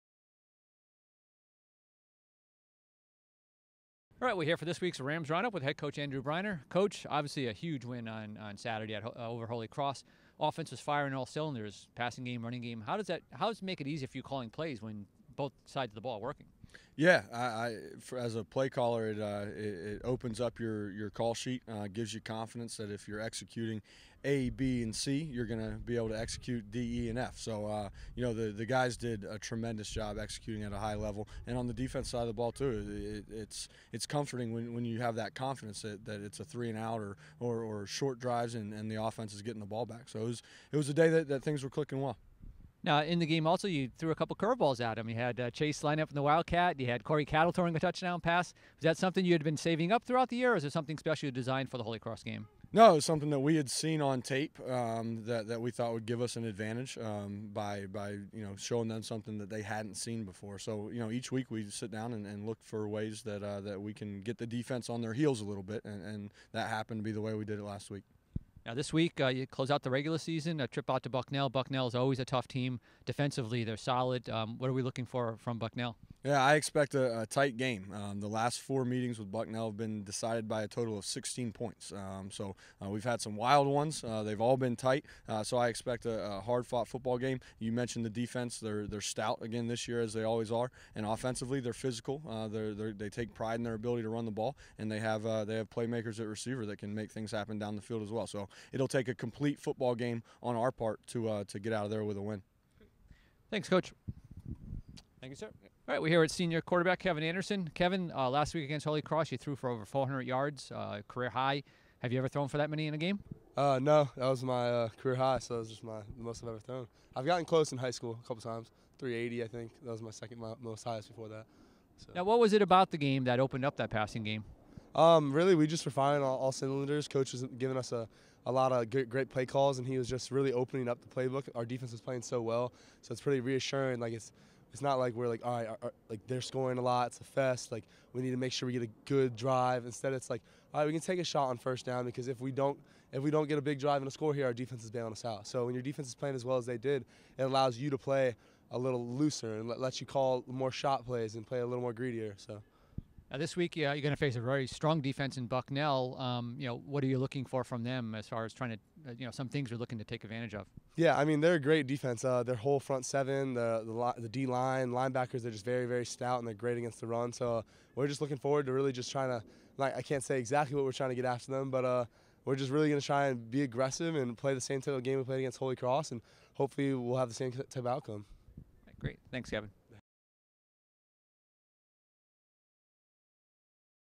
Ram Roundup 2016 Football Ep. 9
Recap of Fordham's win over Holy Cross in the Ram Crusader Cup and a preview of Saturday's regular season finale at Bucknell with head coach Andrew Breiner and senior Kevin Anderson.